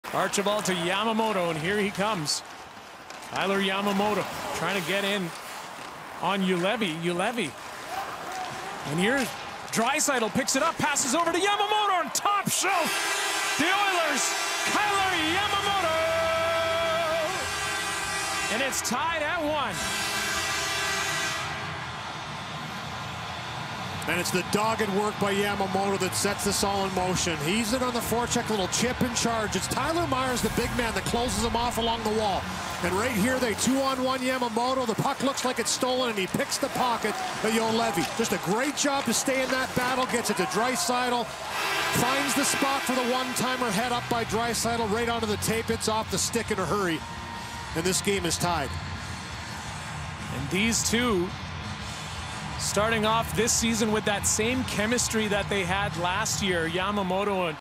Kailer Yamamoto Scores One-Timer Off Feed From Leon Draisaitl
Edmonton Oilers forward Kailer Yamamoto scores a great one-timer goal after a feed from Leon Draistaitl to tie up game against Vancouver Canucks.